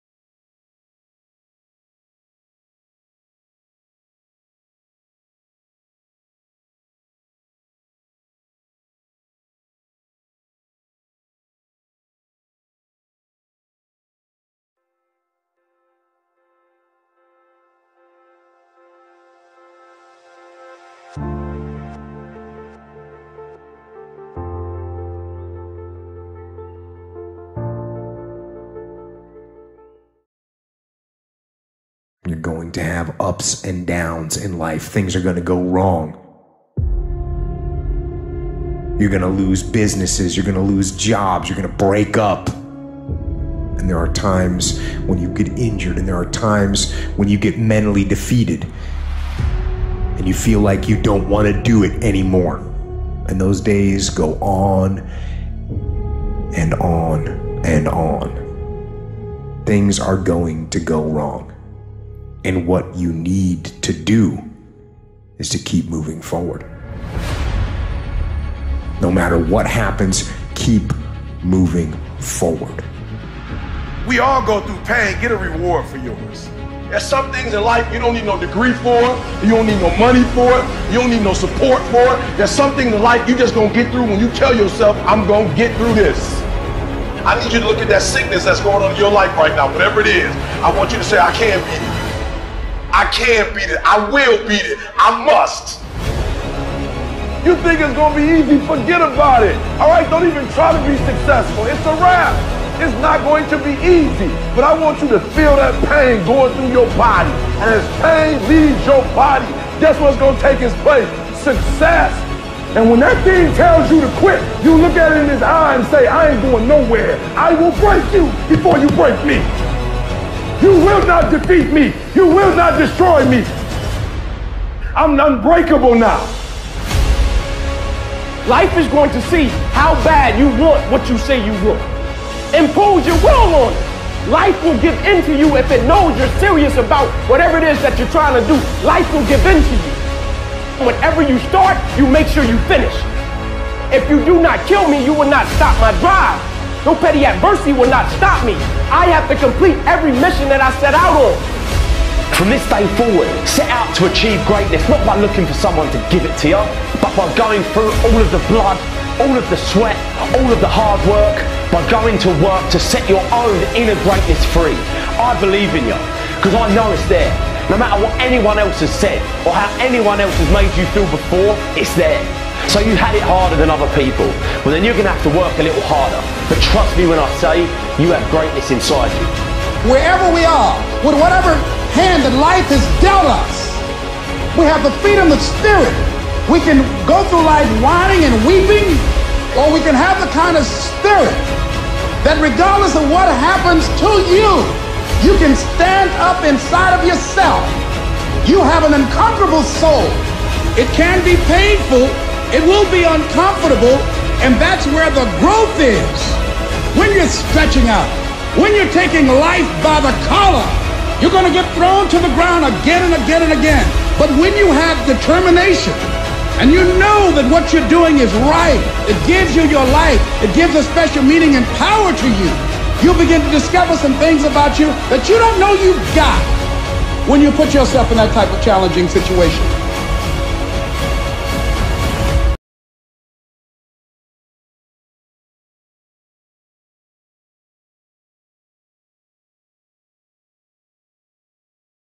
What You Have To Do To Set Your Own Inner Greatness Free. Motivational Advice for 2020
I hope that this video will make you start a day with a grateful heart that light illuminates from within.

By becoming consciously aware, I have attained a certain level of stability and mastery over my life. So, I have this desire to help people expand their awareness and get unstuck to reach their potential.

Feel free for caption & translate this video!